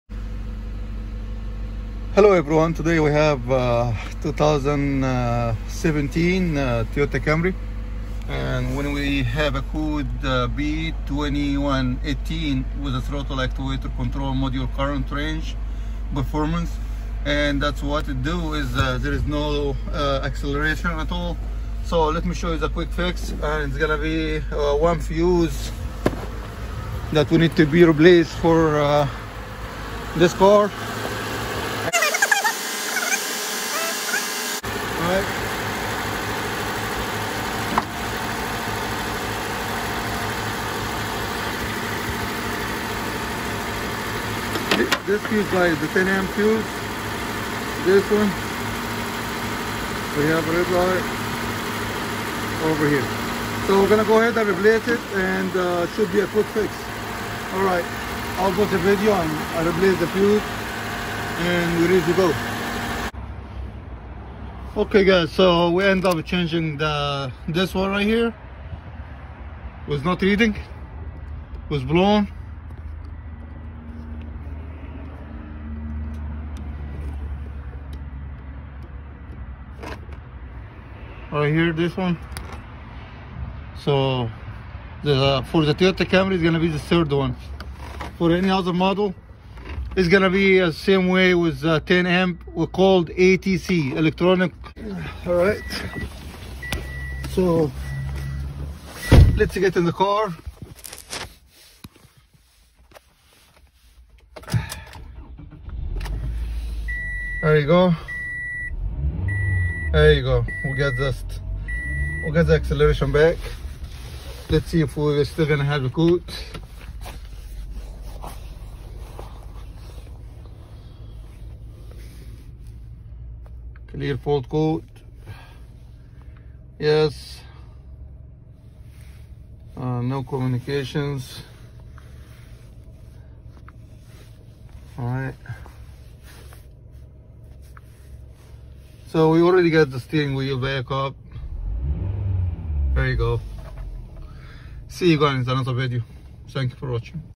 Toyota P2118, Throttle Actuator Control Motor Current Range/Performance
P2118 Toyota etc Throttle actuador control no acelera
Toyota P2118, rango / rendimiento de corriente del motor de control del actuador del acelerador

Toyota no acelera por baja de tensión de batería o por desconectar la batería ,  al caer la tensión , aumenta el doble la corriente y quema el fusible.  código p2118 , solo reemplazar fusible etcs , fácil , algo bueno para no perder tiempo .  a veces nos vamos a lo mas difícil y es algo simple que podemos descartar en un diagnostico preliminar , saludos .. Luis Arleo , después reinicar con el scanner , o esperan el próximo monitoreo , y apaga la luz mil sola

Toyota P2118, Диапазон / производительность двигателя управления приводом дроссельной заслонки
豐田 P2118，油門執行器控制電機電流範圍/性能